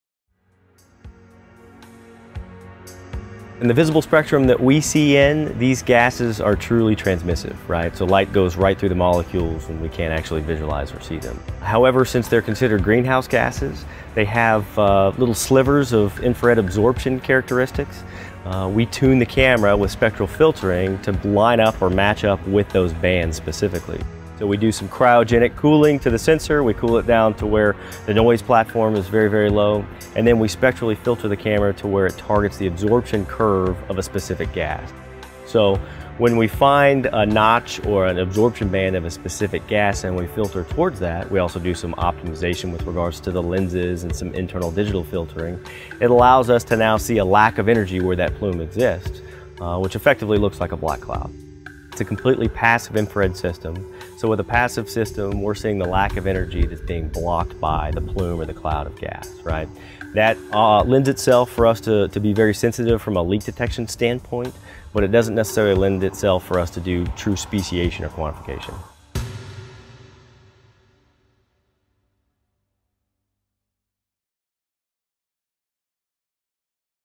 How does Optical Gas Imaging Work?
How does Optical Gas Imaging work? Why do you need a special thermal camera to visualize gases?